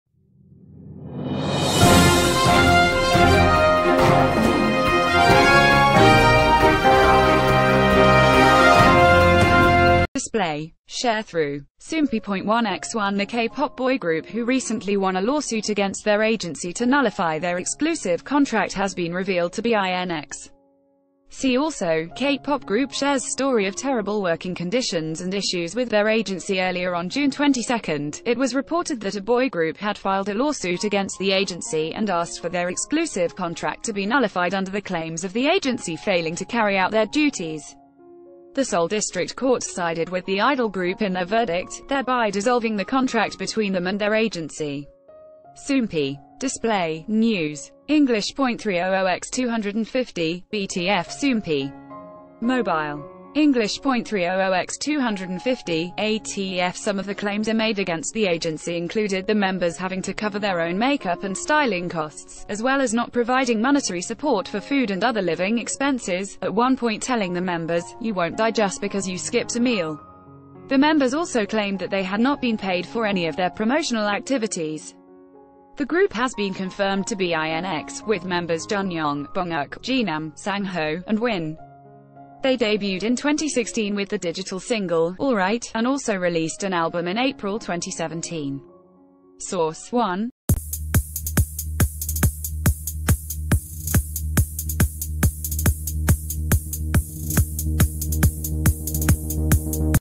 K-Pop Group Involved In Lawsuit Against Agency Over Working Conditions Revealed To Be INX
Display. Sharethrough. Soompi.1x1 The K-pop boy group who recently won a lawsuit against their agenc...